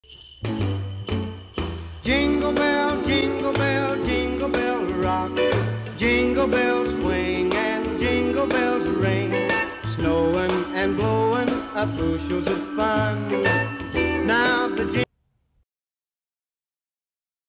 Bighorn Sheep Jingle Balls
Bighorn Sheep in Badlands National Park playing Jingle Balls!